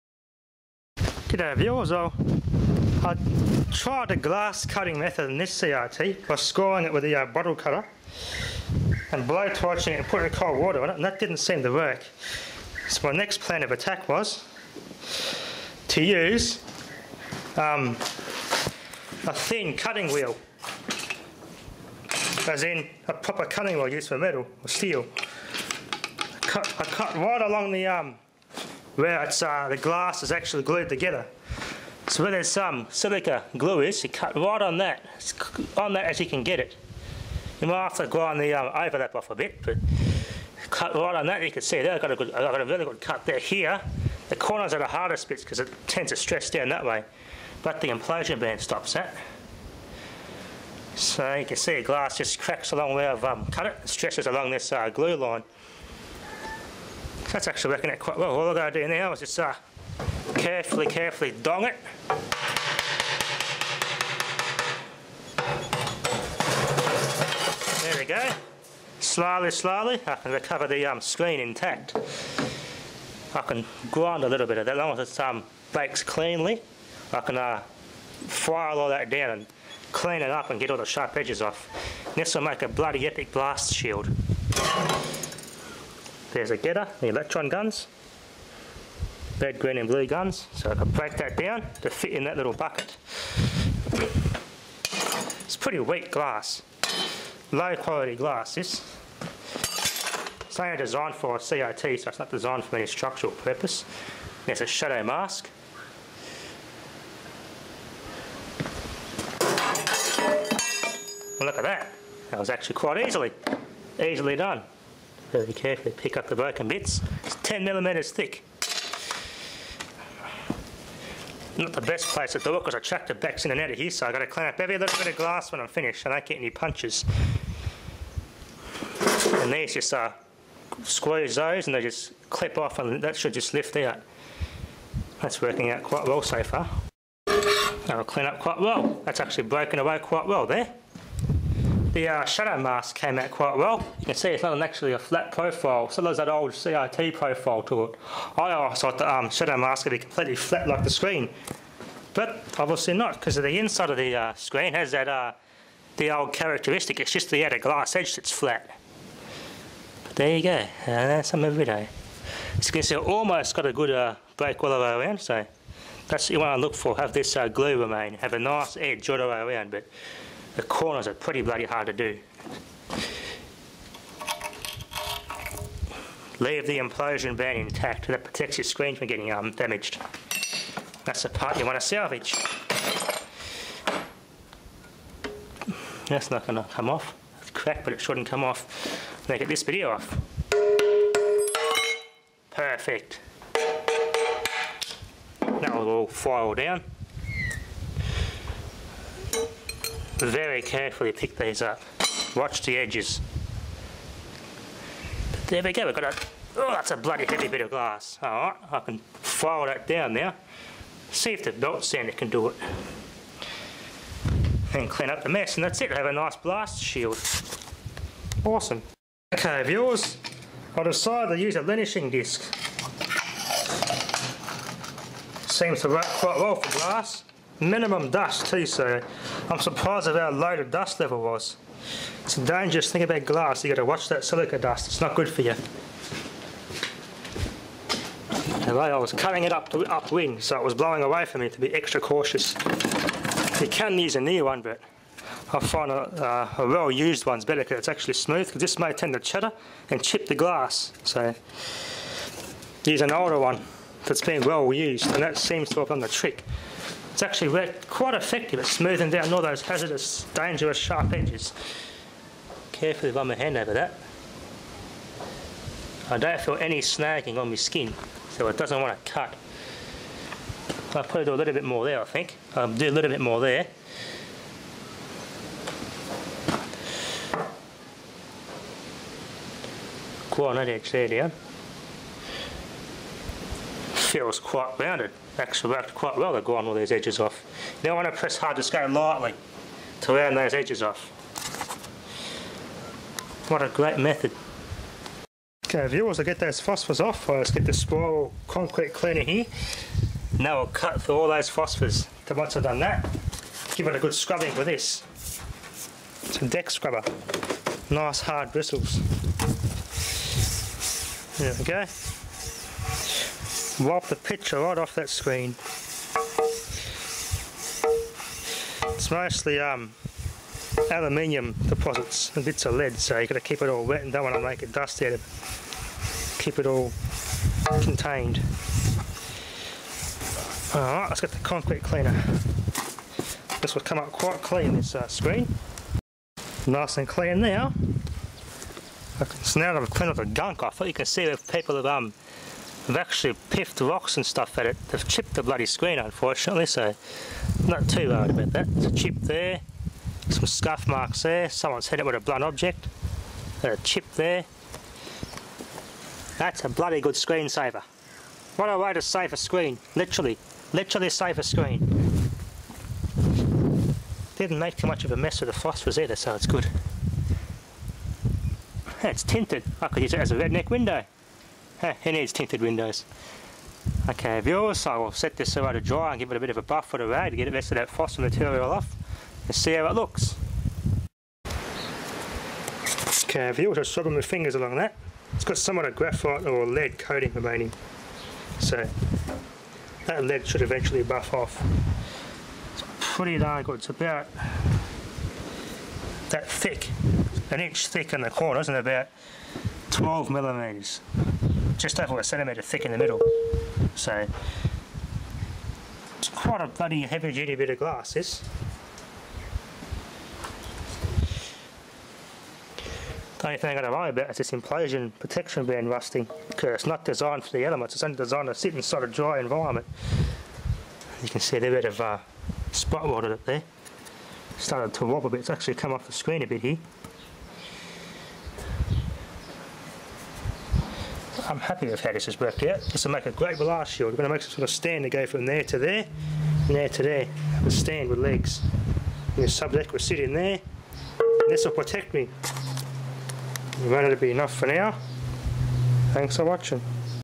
TV Screen Blast Shield
This should work quite well to protect me and the camera from debris!